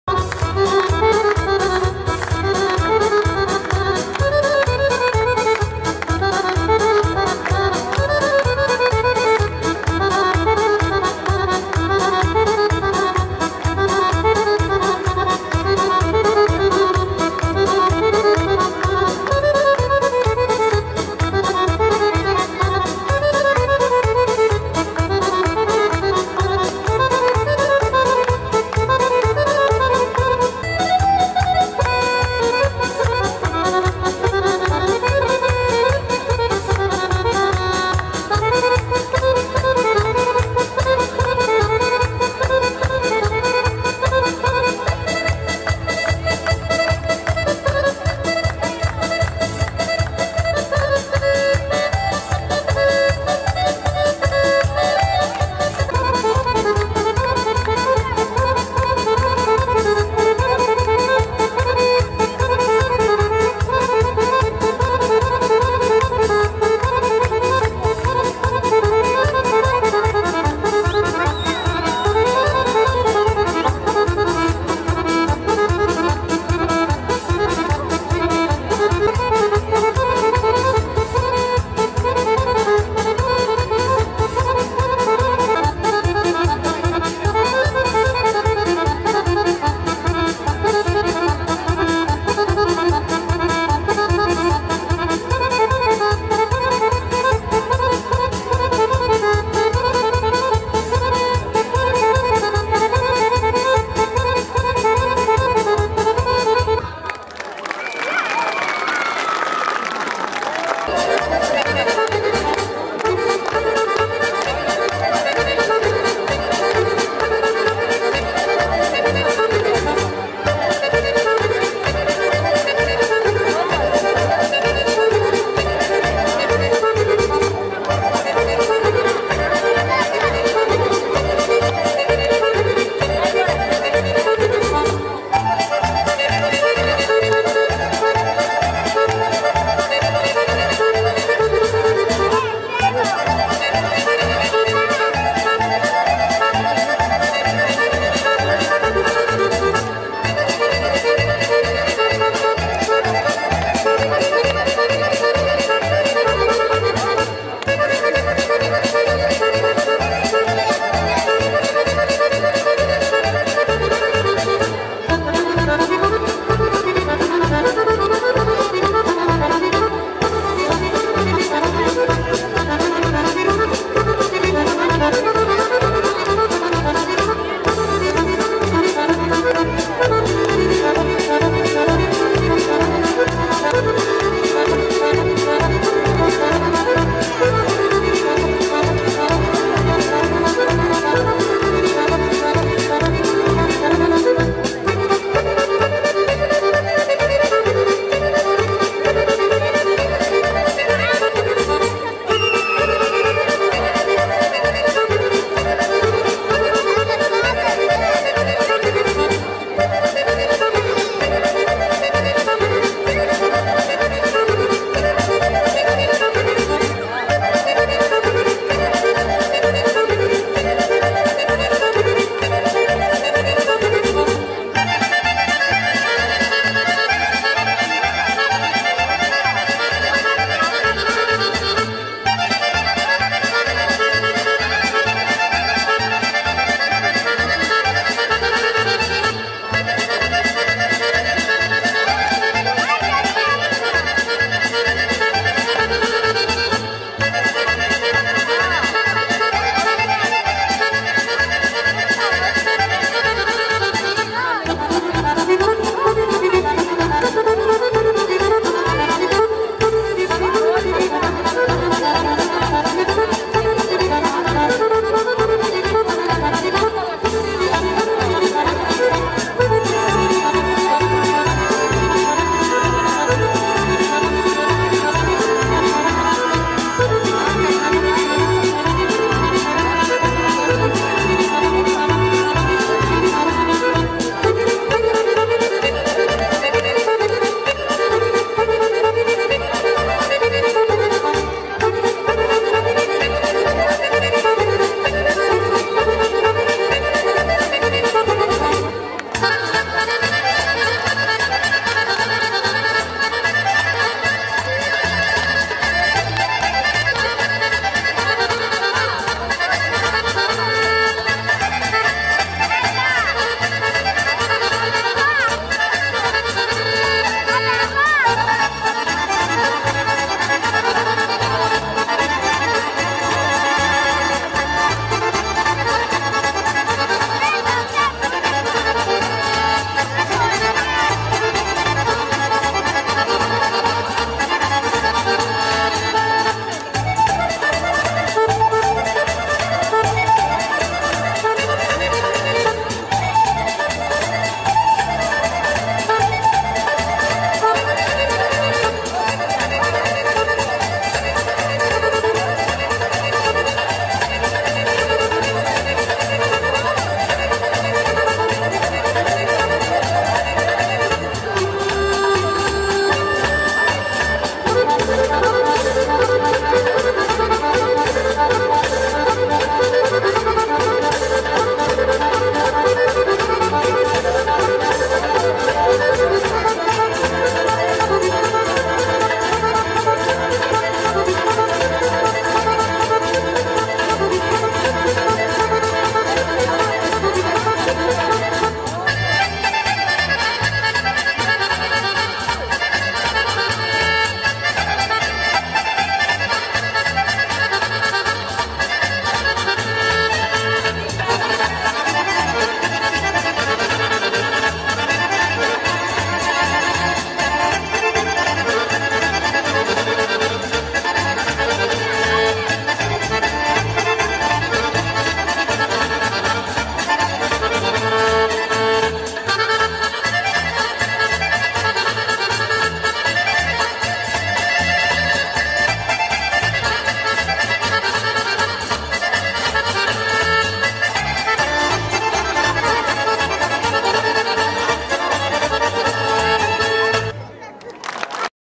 GFA Srebrenik , Berovka-Ratevka
Gradski teatar Srebrenik, podliježe autorskim pravima / GTS, GFA/
Gradski Folklorni Ansabml-Srebrenik ( u repertoaru ansambla od 2006 g.)
Izvedba : Međunarodna smotra folklora Maoča-Brčko 2010 g.                                               Koreograf: Edvin Sadiković
Makedonska nacionalna igra: Berovka-Ratevka